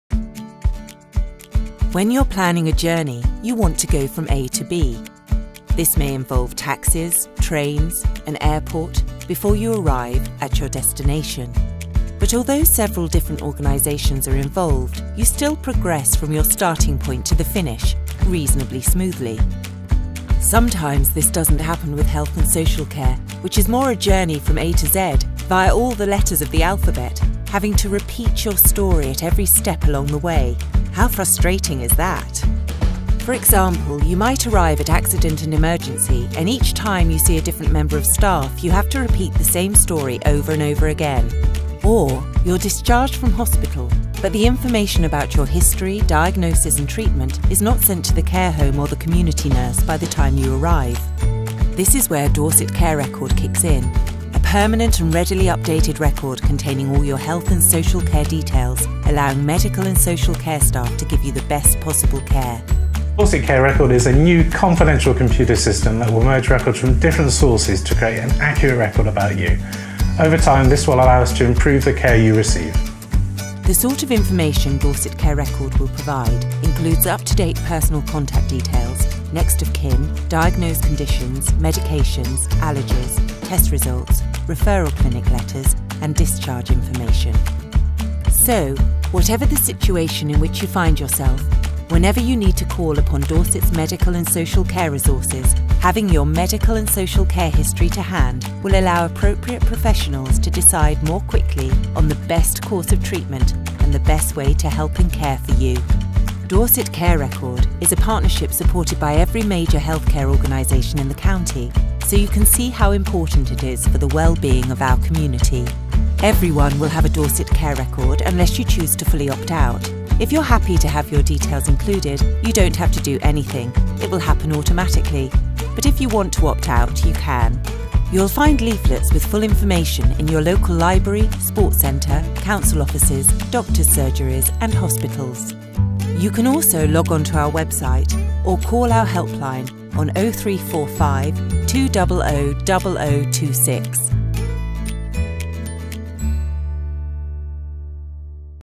Dorset Care Record
All you need to know about the Dorset Care Record and how it will provide joined-up healthcare in Dorset for everyone